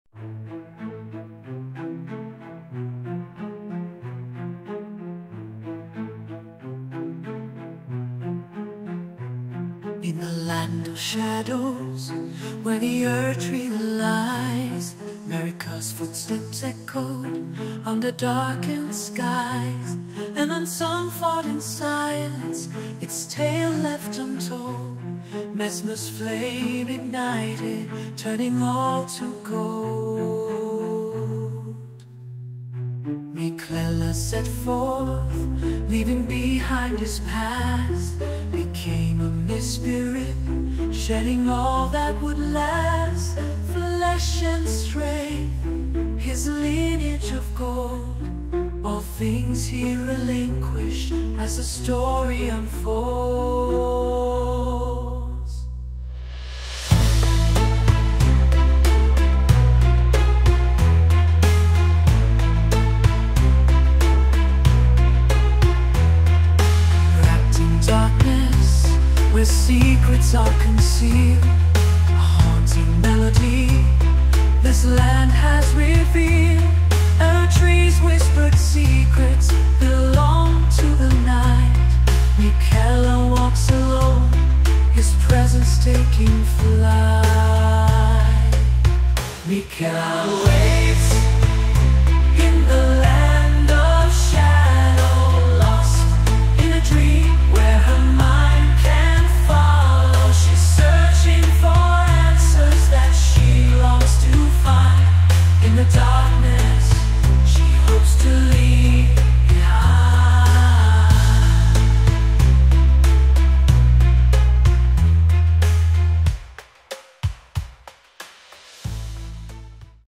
I ASKED AI TO SING A SONG About ELDEN RING DLC!! SHADOW OF THE ERDTREE!
I used Suno to create it.